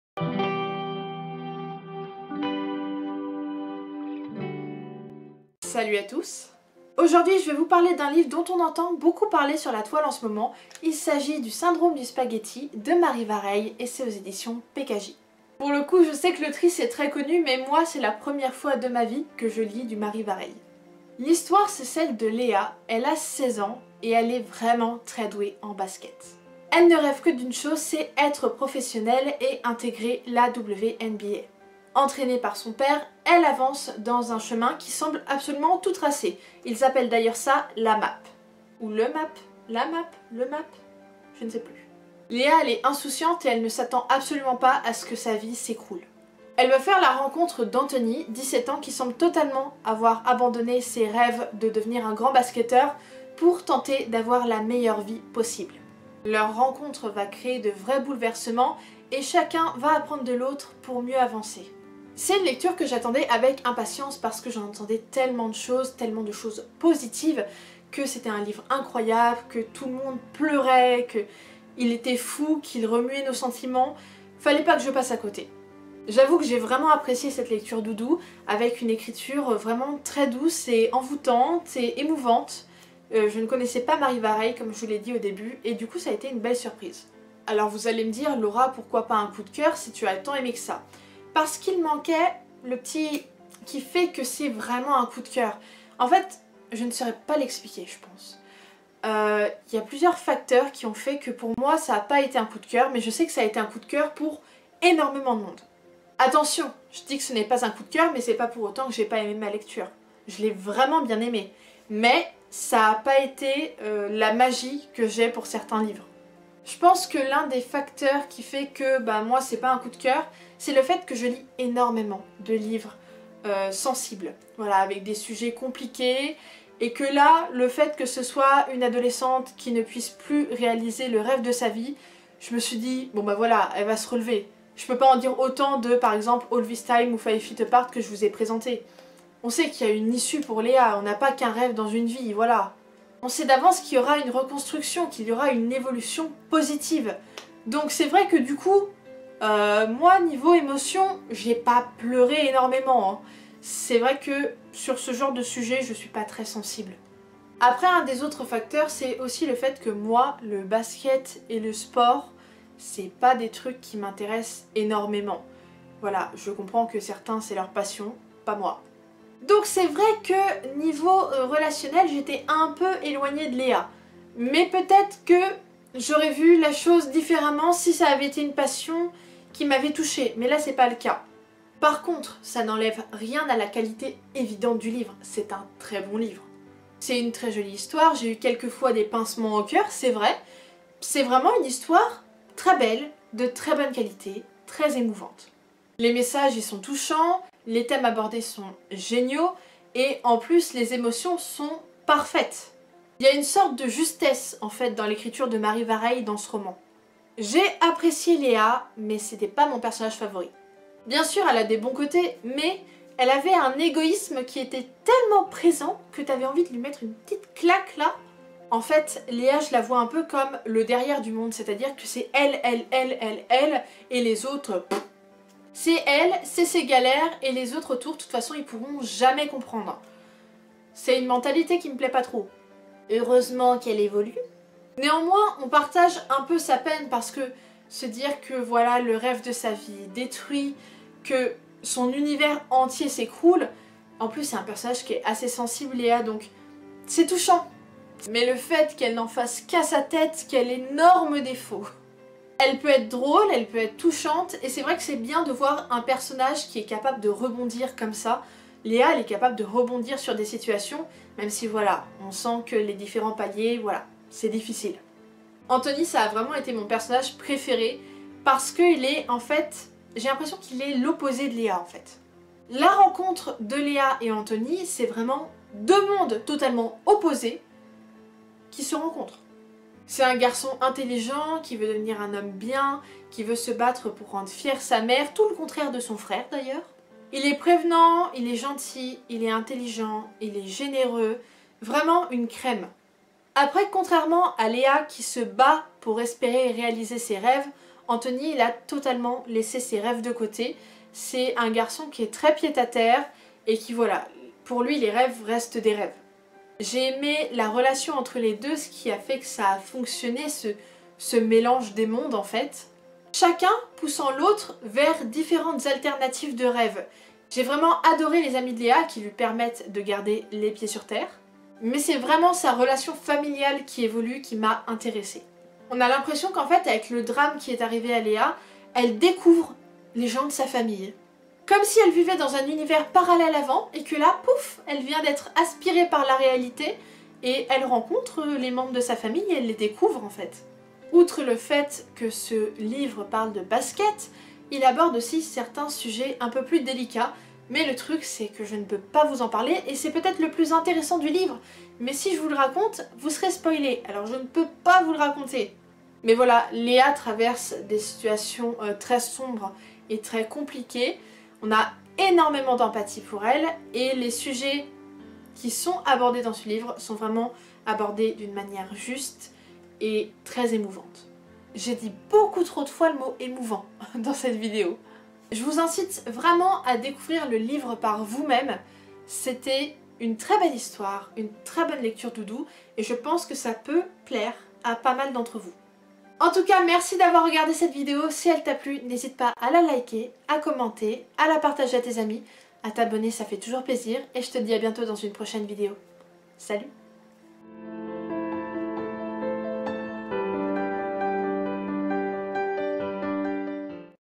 "Le Syndrome du Spaghetti" 🍝 - Critique Livre
Salut à tous ! Aujourd'hui on parle d'un livre dont vous avez probablement entendu parler vu le succès sur bookstagram : "Le syndrome du spaghetti" de Marie Vareille aux éditions PKJ !

LE LIVRE :
"Le Syndrome du Spaghetti" (Pocket Jeunesse) de Marie Vareille